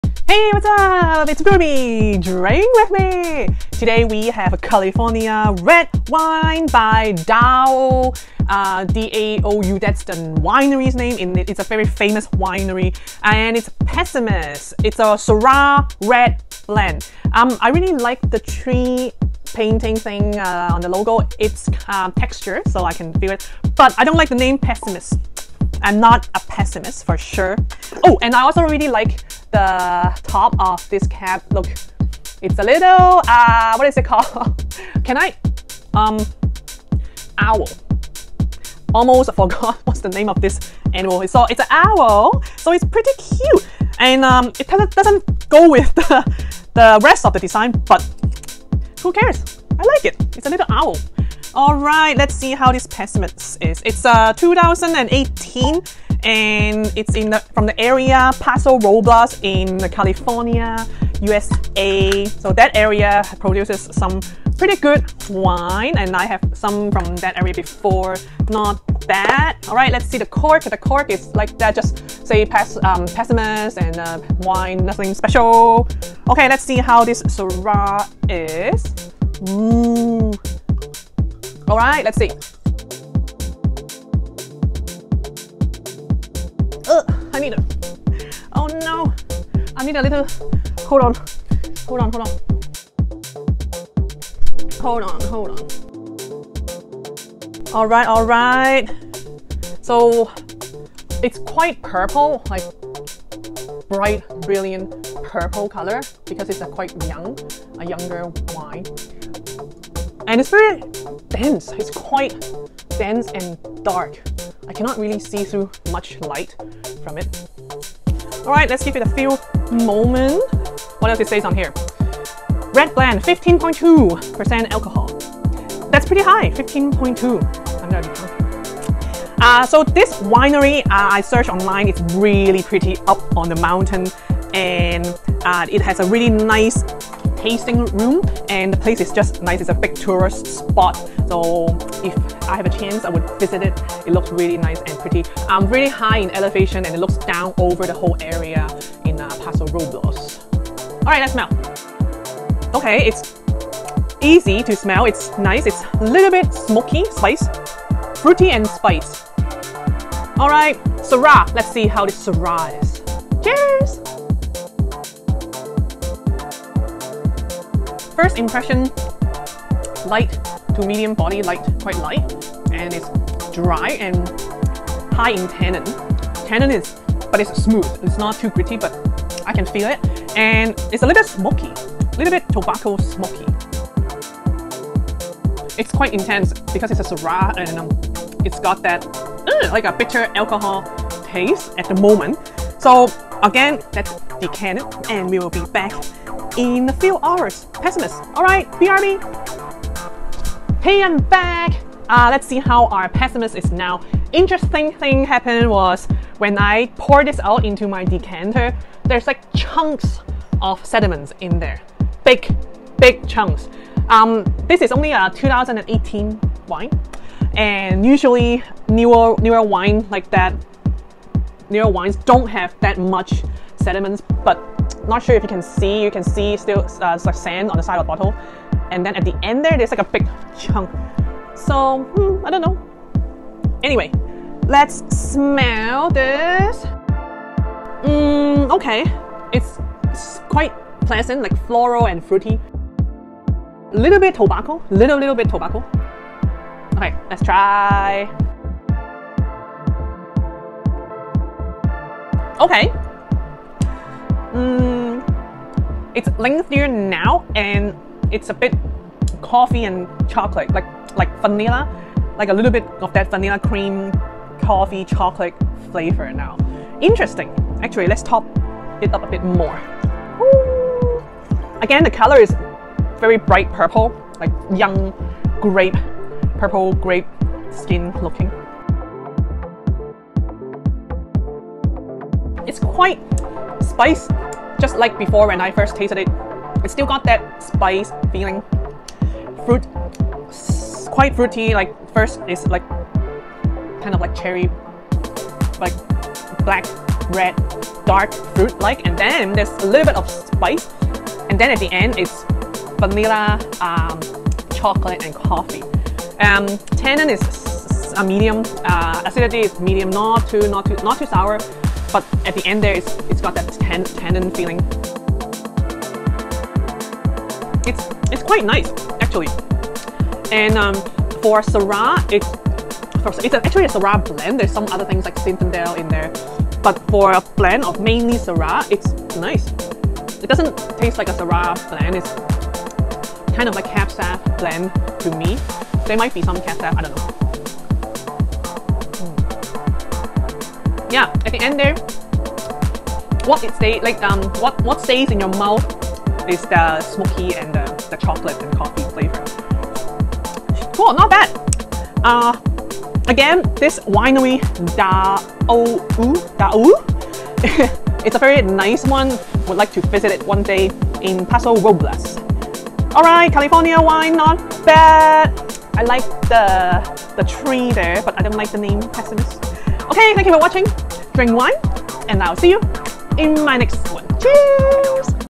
California Red - Paso Robles - The Pessimist 2018, DAOU, Wine Tasting in Black Tights / Nylons
Hey what's up it's Ruby! DRINK WITH ME! Join me for the tasting of this 2018 The Pessimist by DAOU, Paso Robles, Central Coast, California, USA. Red wine tasting in shiny black tights nylons. I hope you like this video! And I will see you *SNAP* in my next one!

"In a pursuit to create delicious wine inspired by varieties that flourish in Paso Robles, The Pessimist is a blend of Syrah, Petite Sirah, Zinfandel and Tannat. Sourced and crafted by the DAOU Vineyards winemaking team, this wine demonstrates classic old world style with the fruit-forward characteristics of California."

Products:
Hosiery - Cecilia de Rafael Eterno 20
Wine Glass - Riedel Winewings Cabernet Sauvignon
Decanter - Riedel Performance